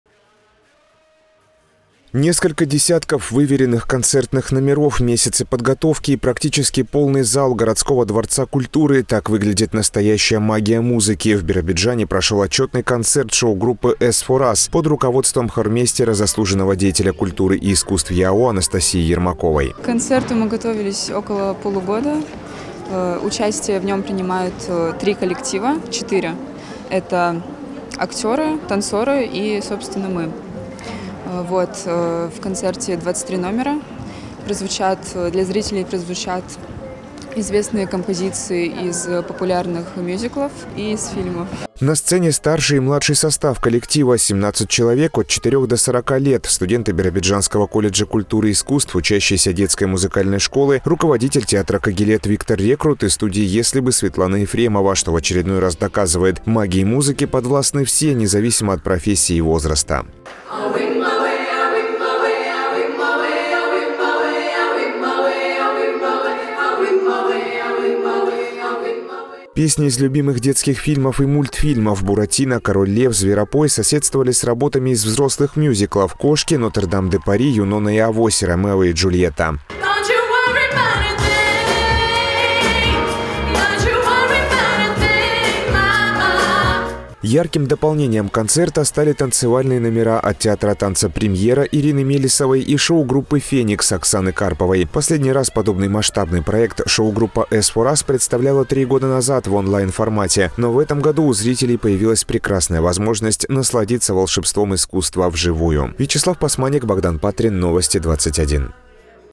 Концерт шоу-группы «As for us» состоялся в городском Дворце культуры Биробиджана
Несколько десятков выверенных концертных номеров, месяцы подготовки и практически полный зал городского Дворца культуры – так выглядит настоящая «Магия музыки» - в Биробиджане прошёл отчётный концерт шоу-группы «As For Us» под руководством хормейстера, заслуженного работника культуры ЕАО Анастасии Ермаковой.